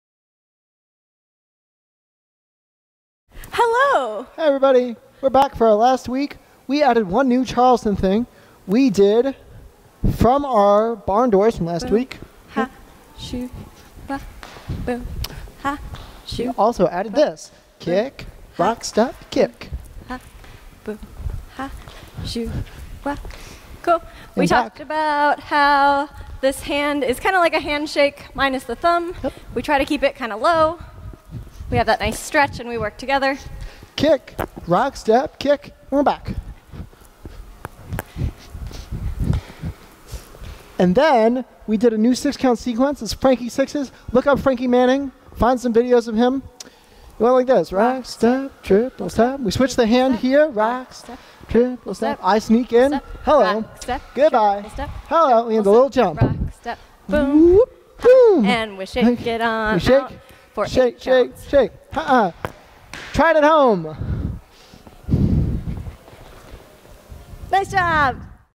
Level 1C - 6-Count and Charleston - Week 4
with Samantha Nguyen and Isaac Garfinkle